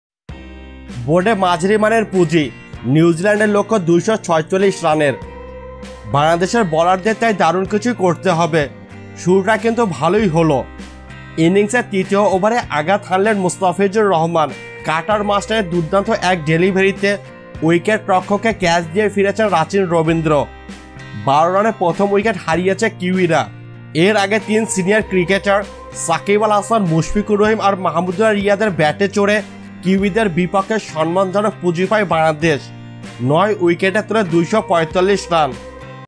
মোস্তাফিজের দুর্দান্ত ডেলিভারিতে শুরুতেই উইকেট তুলে নিলো বাংলাদেশ! Ban vs NZ
ICC World Cup || Bangladesh vs New Zealand || Ban vs NZ || Sports 24 || Mustafizur Rahman